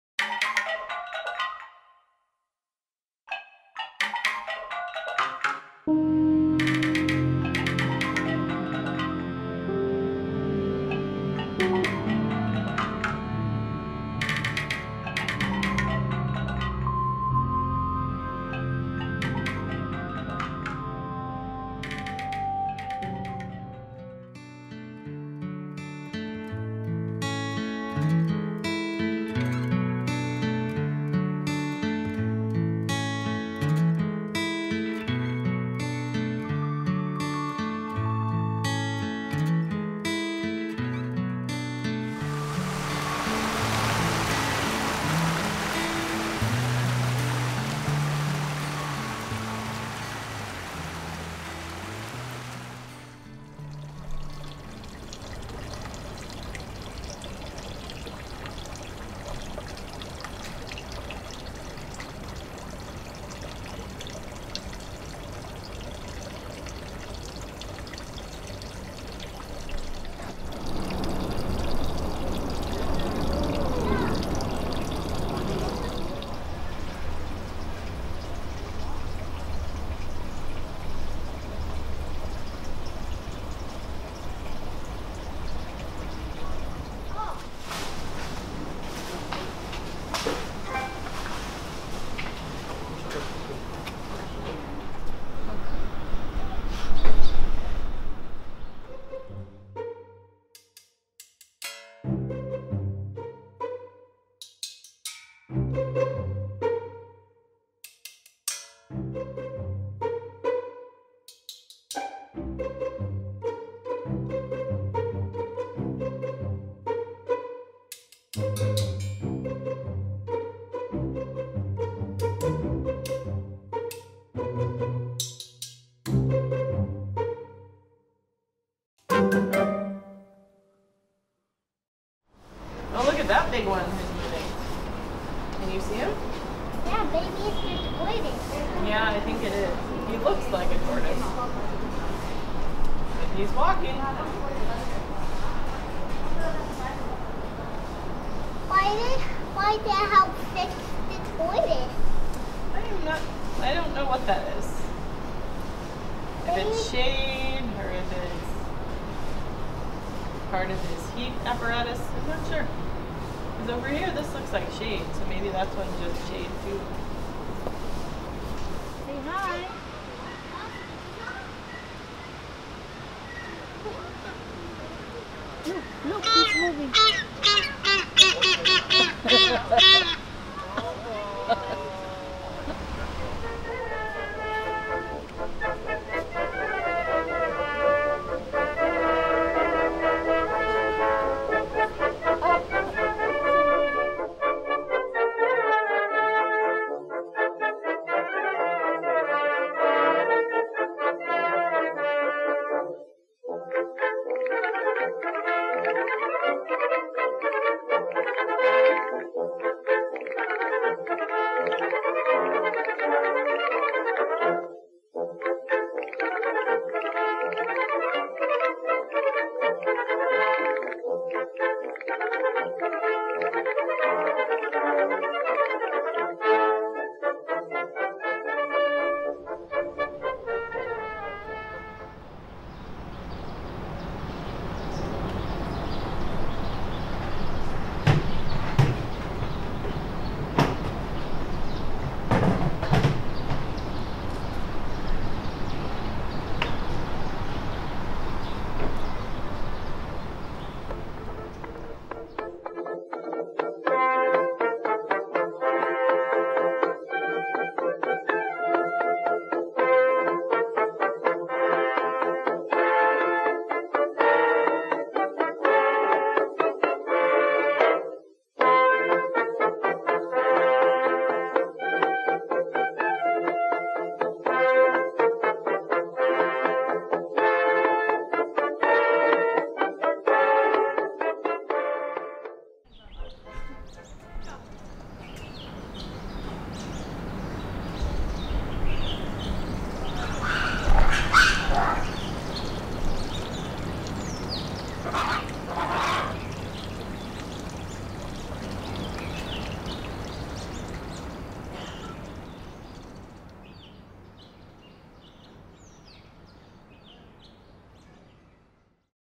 City Animals
CITY ANIMALS: The creatures of Hong Kong come together in the city zoo. The short film shows the contrast between the busy humans in the city centre and the peaceful turtles, lively birds and playful monkeys at the zoo. At the same time, if you look closely, the film also shows many similarities between the creatures.
Date: Feb 15-16, 2017
Location: Hong Kong Zoological and Botanical Gardens, Central
Shot by: Nikon D5500, Panasonic GH4
Edited by: Premiere Pro CC 2017